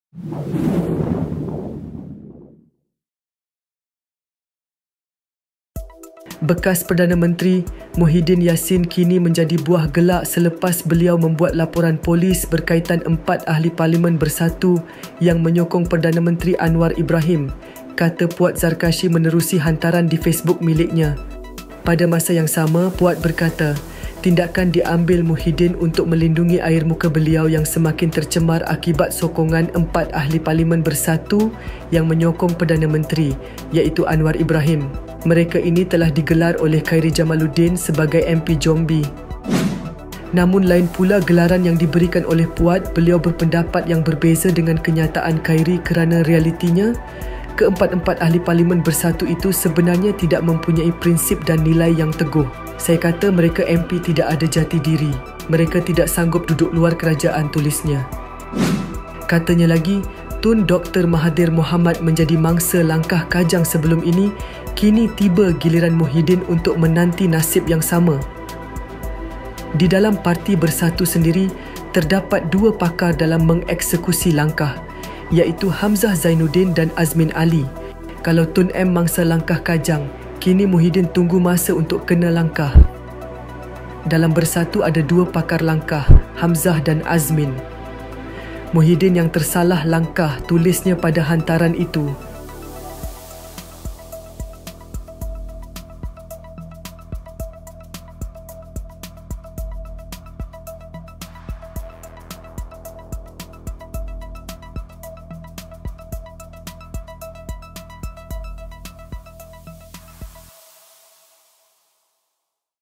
MUHYIDDIN Lapor Polis Berkaitan Empat Ahli Parlimen Bersatu! Tengok Reaksi Puad Zarkashi!
Bekas Perdana Menteri, Muhyiddin Yassin kini menjadi buah gelak selepas beliau membuat laporan polis berkaitan empat ahli parlimen yang menyokong Perdana Menteri Anwar Ibrahim, kata Puad Zarkashi menerusi hantaran di Facebook miliknya.